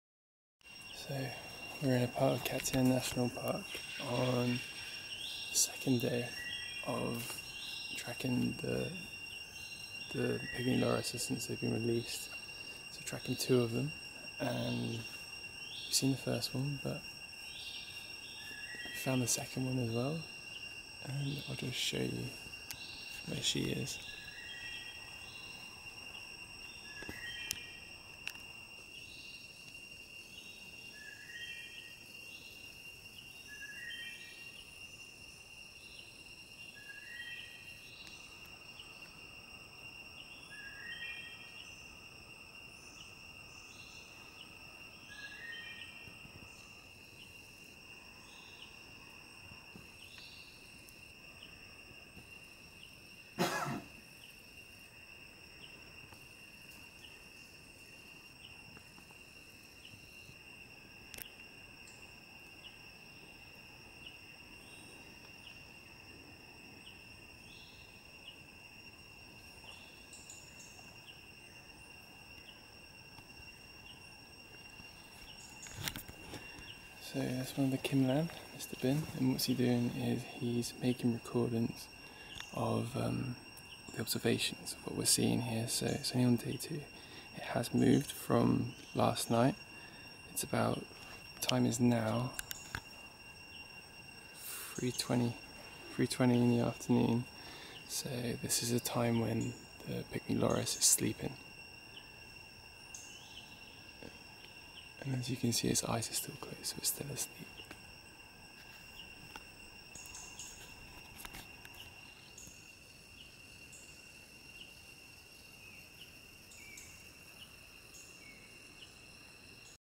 Looking for Loris Part 3 - FOUND THEM!
Now the loris are back in the forest, our post release team are monitoring their progress using radio-tracking equipment - collecting vital data on their travel movements, sleep sites, and tree preference. David was lucky enough to see the loris after their second night of freedom, when he went with Binh  - the head of our Post-Release Team - to the release site in Cat Tien National Park.